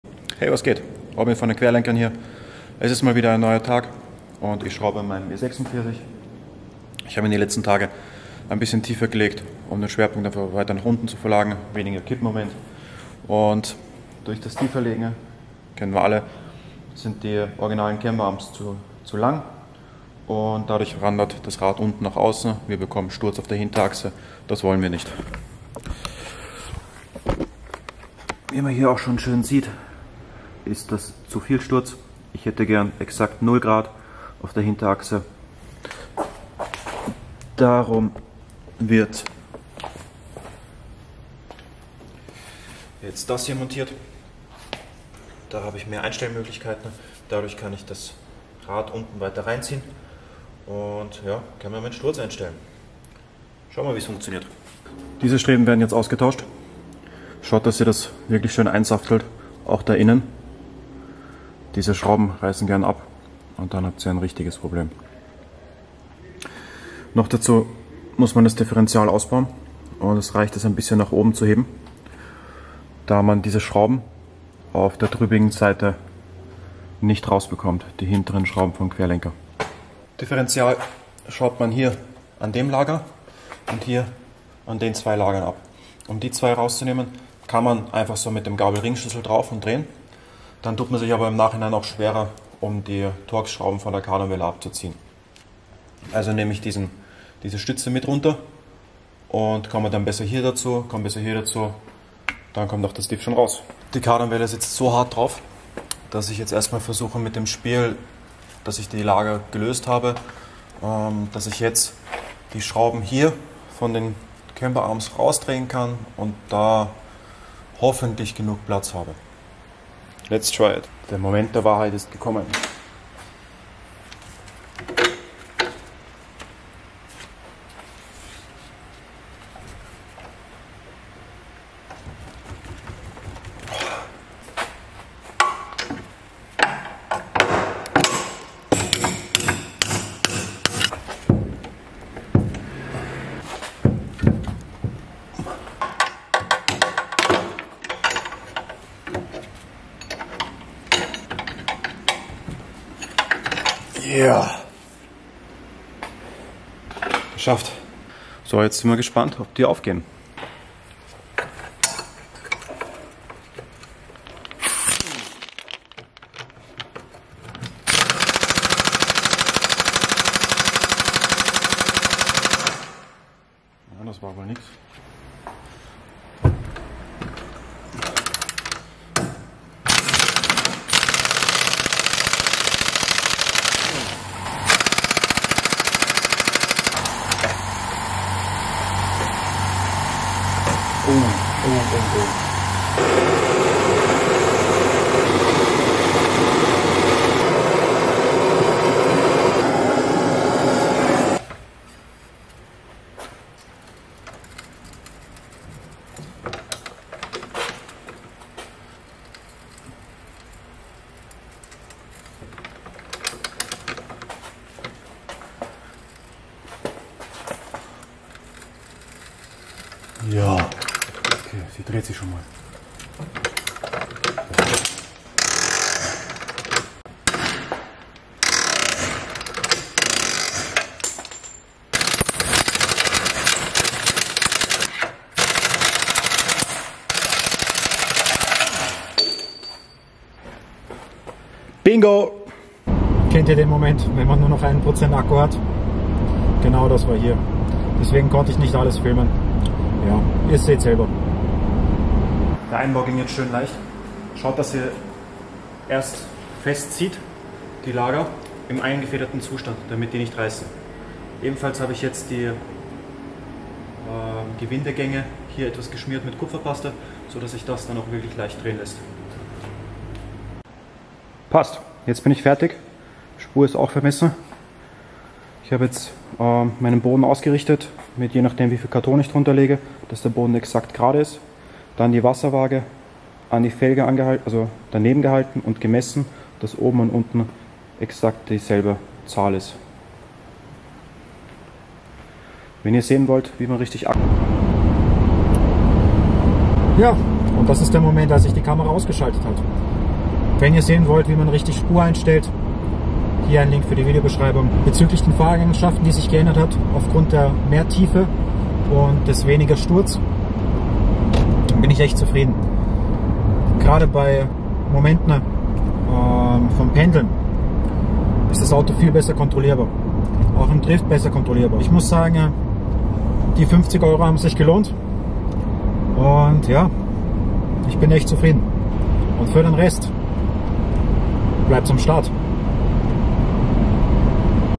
Camber Arms Drift BMW E46 Sturzversteller Einstellbare Querlenker
Mein Drift E6&Pfirsich bekommt auf der Hinterachse ein Upgrade um noch mehr Performance abzuliefern